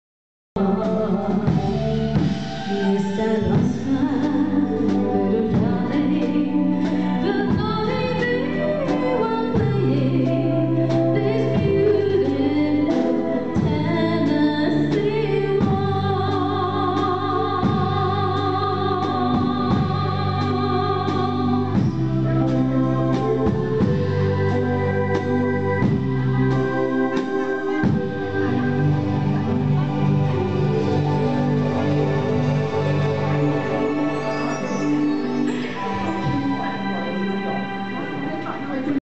Become1_52.avi
A&D Live Band Show - 6
with Become One Band
at
Bravo Dance Studio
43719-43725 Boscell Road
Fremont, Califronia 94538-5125
USA